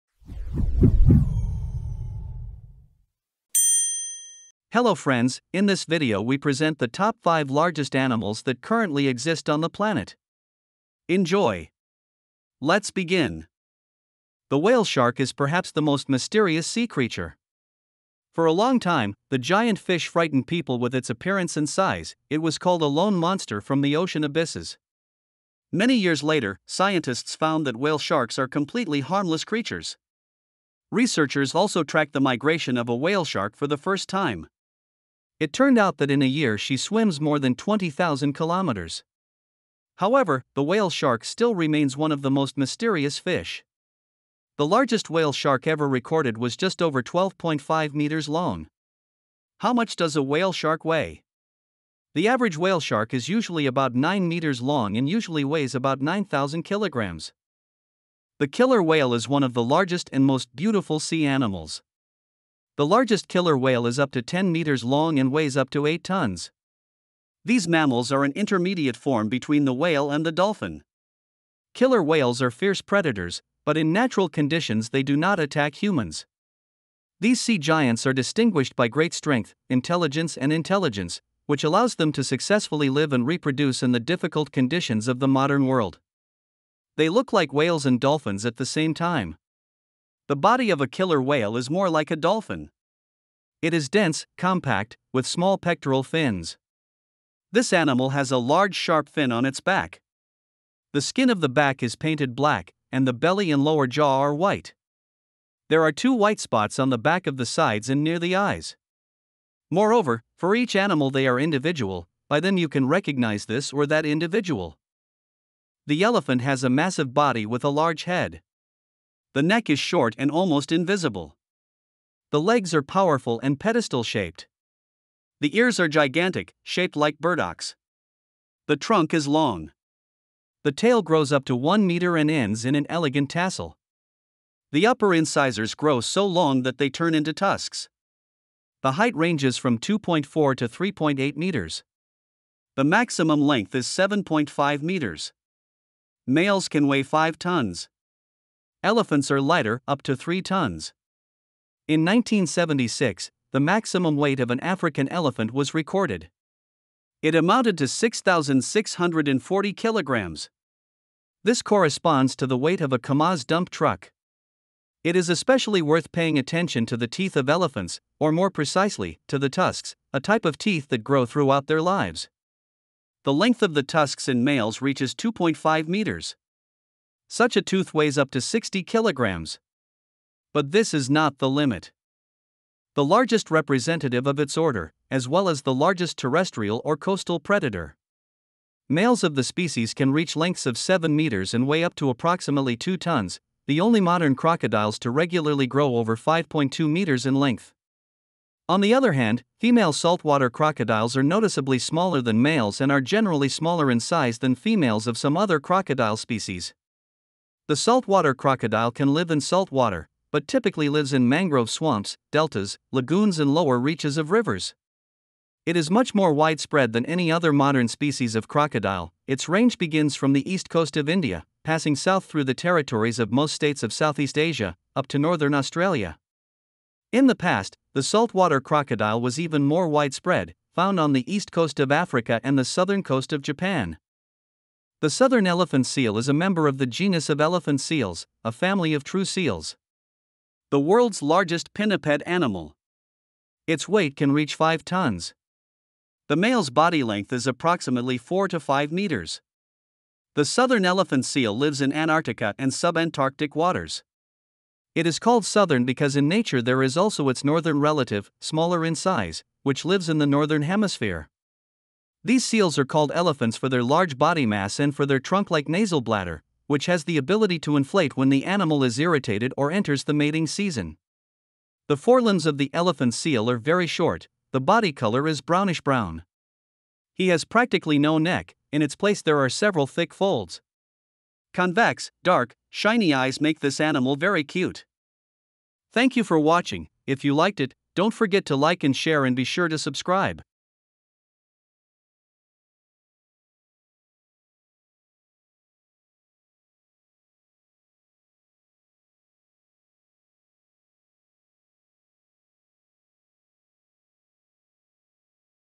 Top 5 largest animals on the planet.
Top 5 largest animals.  Animals come in many shapes and sizes.  Whether you measure by weight, height or length, some animals are huge compared to others in their class.  Here is a Top 5 largest animals in the world